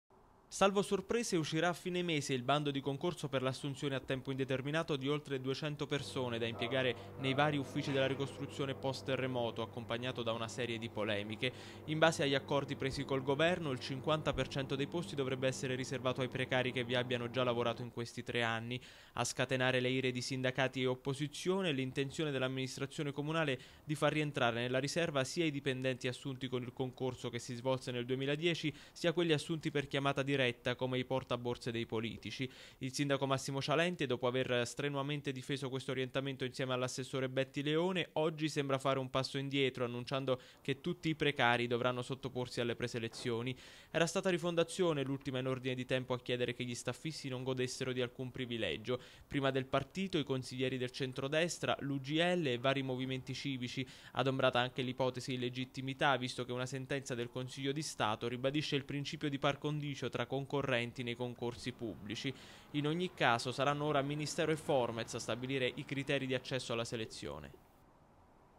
Concorso assunzioni ricostruzione, dietrofront di Cialente: "Preselezione per tutti"
Clamoroso dietrofront del sindaco Massimo Cialente sulle modalità d'accesso al concorso il cui bando sarà pubblicato nei prossimi giorni per l'assunzione a tempo indeterminato di oltre duecento persone da impiegare nei vari uffici della ricostruzione.

Anche i lavoratori che già prestano servizio al Comune dell'Aquila, siano essi stati assunti con il concorso del 2010 che per chiamata diretta, dovranno svolgere le preselezioni per l'assunzione con contratto a tempo indeterminato.

Smentito dunque, almeno a parole, l'orientamento iniziale (manifestato anche ad Abruzzo24ore.tv dall'assessore al Personale Betti Leone) in base al quale i dipendenti precari, compresi i portaborse, avrebbero saltato la preselezione.

Il sindaco, ai microfoni di Abruzzoweb, ha annunciato che "anche i precari che hanno vinto il concorso dovranno fare la preselezione, perchè addirittura dei sindacati hanno sollevato il problema".

Certo, saranno comunque Ministero e Formez a mettere l'ultima parola, ma se da parte dell'amministrazione c'è un ripensamento, non solo c'è da immaginare che alla fine sarà come dice il sindaco, ma evidentemente le critiche sollevate avevano un fondamento.